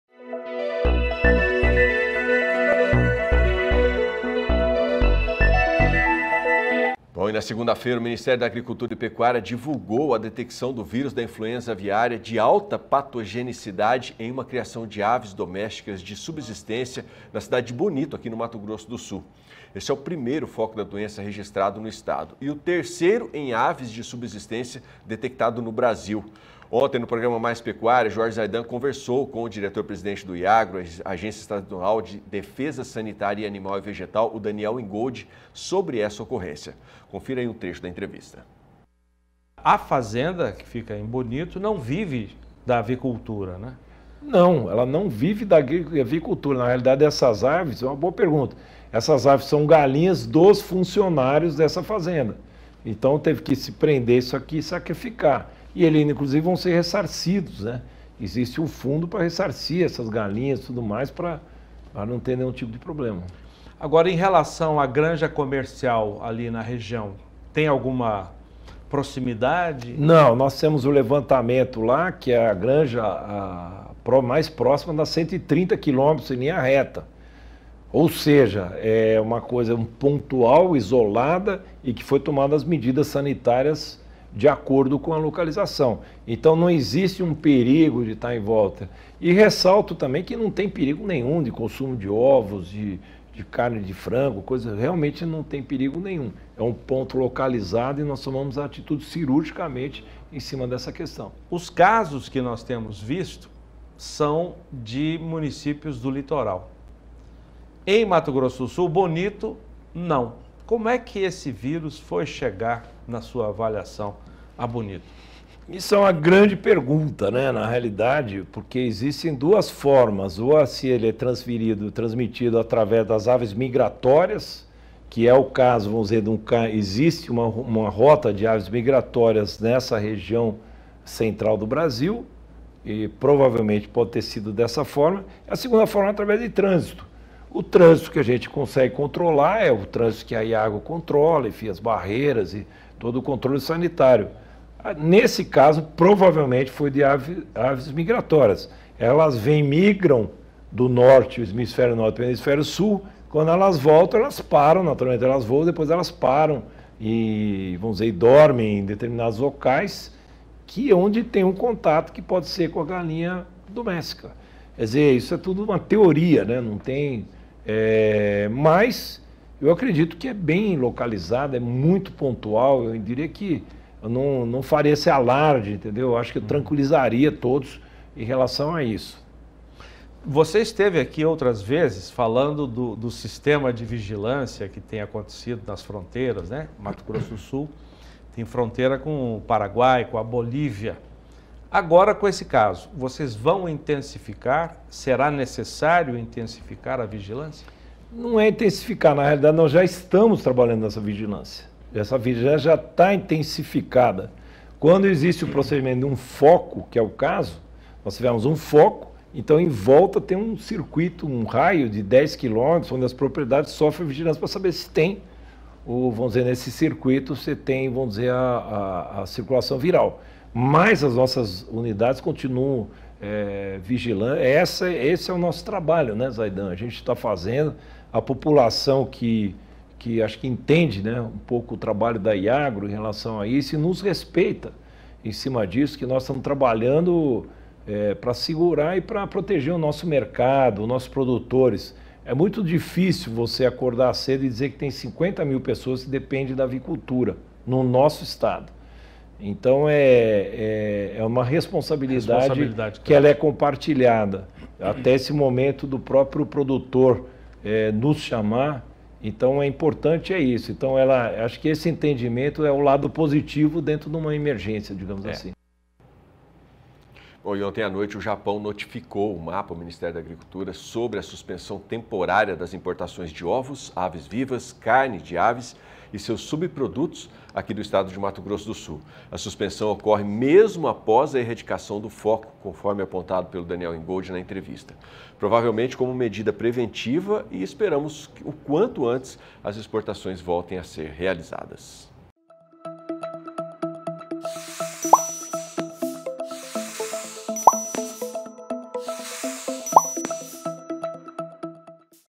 Japão suspendeu importação de carne de aves de MS
O governo japonês suspendeu temporariamente as importações de ovos, aves vivas, carne de aves e seus subprodutos do estado de Mato Grosso do Sul. O Ministério da Agricultura e Pecuária (Mapa) foi notificado, nesta terça-feira (19).